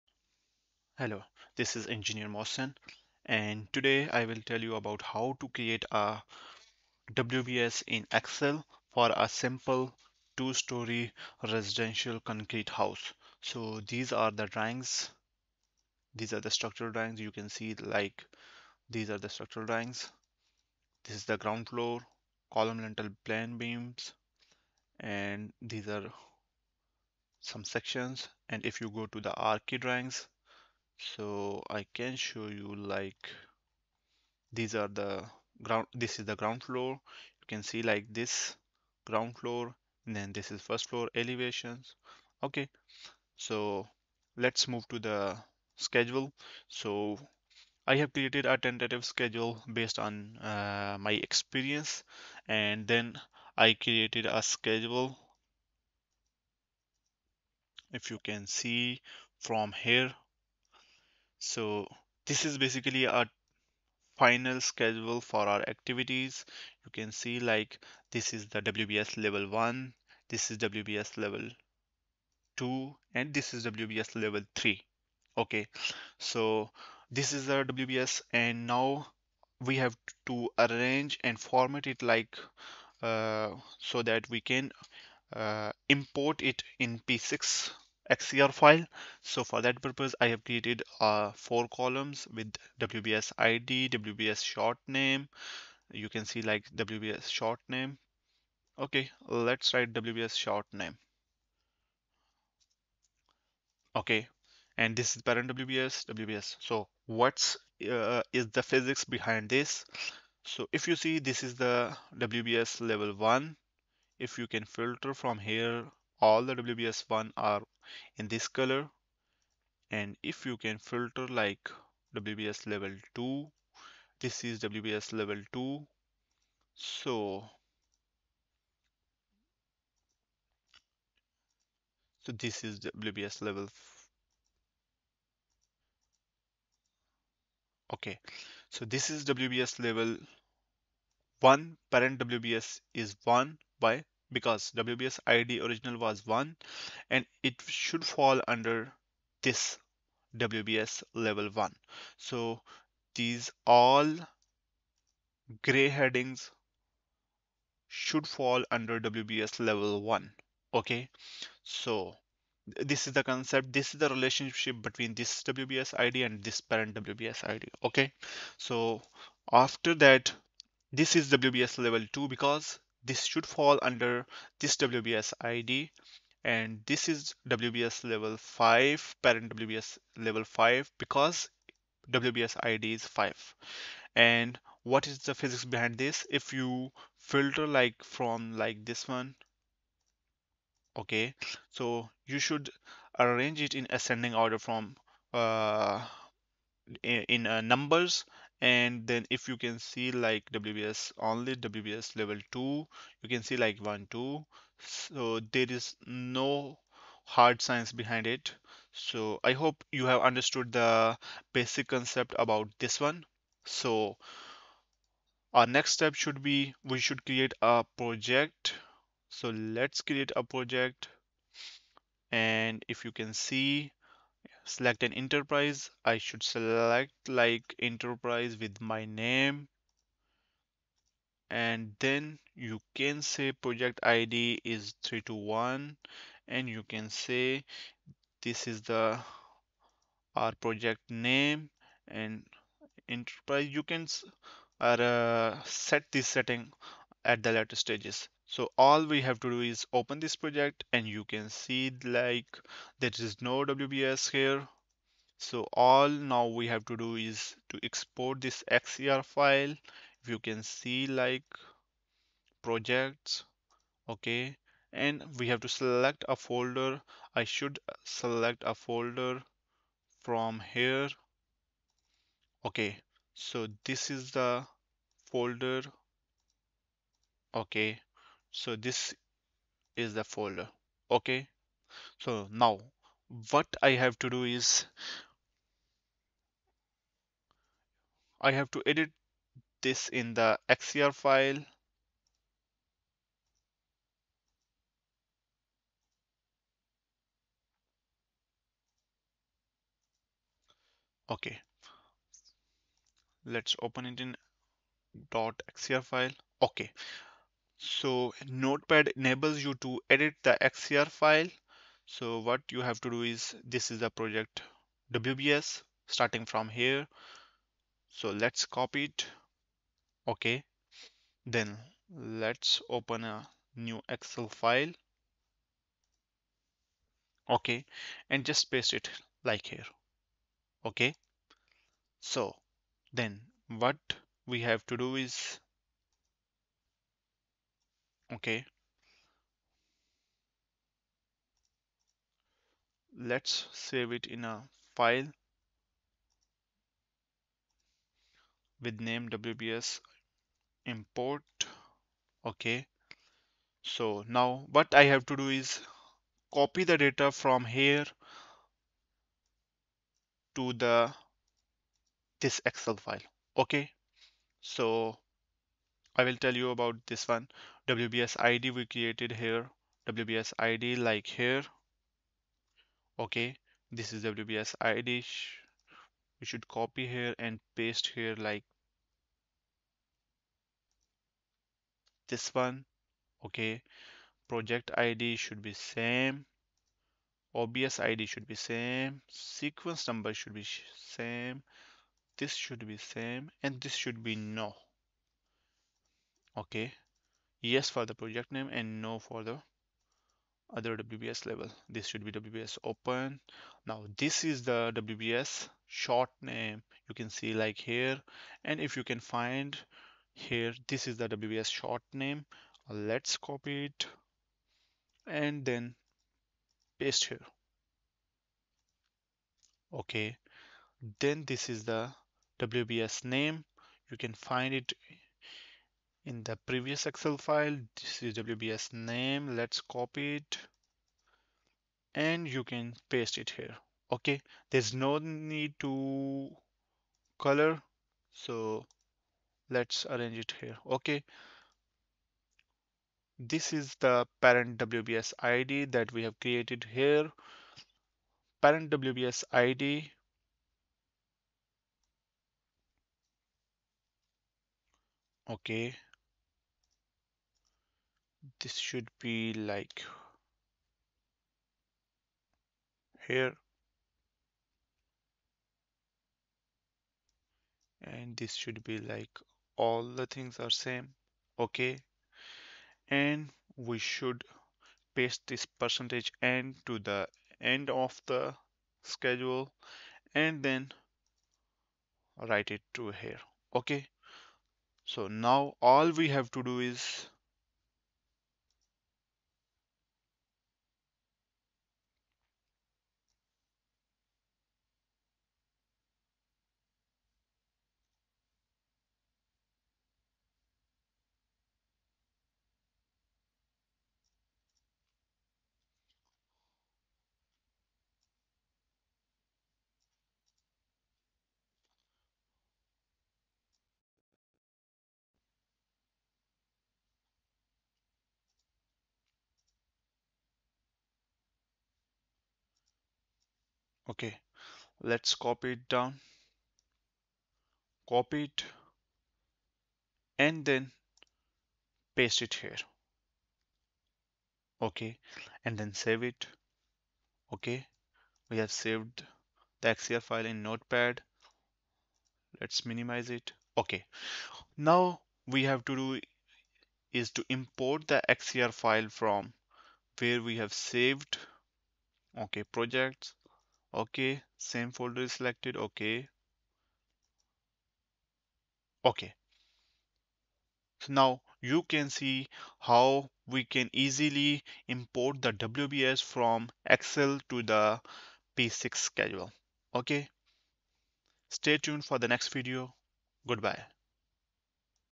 How to Import WBS From Excel to Primavera P6 Professional
In this comprehensive tutorial, we guide you through the process of importing Work Breakdown Structures (WBS) from Excel into Primavera P6 Professional seamlessly. Whether you're new to Primavera P6 or looking to streamline your project management workflow, this video provides detailed instructions and tips to help you efficiently transfer WBS data. From formatting your Excel file to mapping fields in Primavera P6, we cover every aspect of the import process to ensure a smooth transition of your project hierarchy. Follow along as we demonstrate best practices and troubleshoot common issues, empowering you to leverage the full potential of Primavera P6 for your project management needs. Don't let data migration complexities hold you back – master the art of importing WBS from Excel to Primavera P6 with confidence and precision.